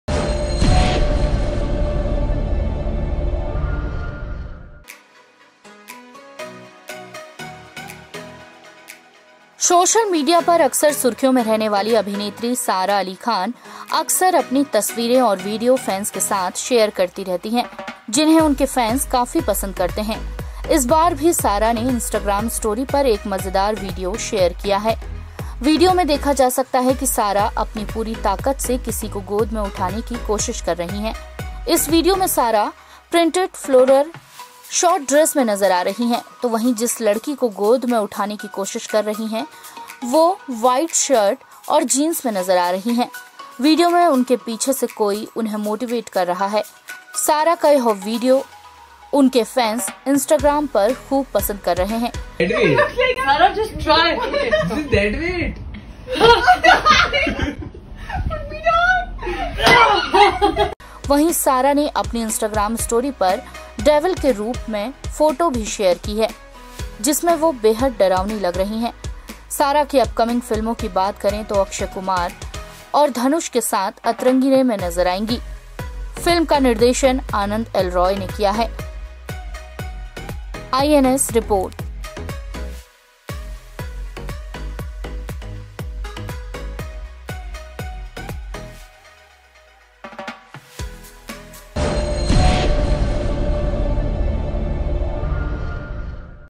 Sara Ali Khan lifts her bestie with both hands, watch video
सोशल मीडिया पर अक्सर सुर्खियों में रहने वाली अभिनेत्री सारा अली खान अक्सर अपनी तस्वीरें और वीडियो फैंस के साथ शेयर करती हैं , जिन्हें उनके फैंस काफी पसंद करते हैं। इस बार भी सारा ने अपनी इंस्टाग्राम स्टोरी पर एक मजेदार वीडियो शेयर किया है।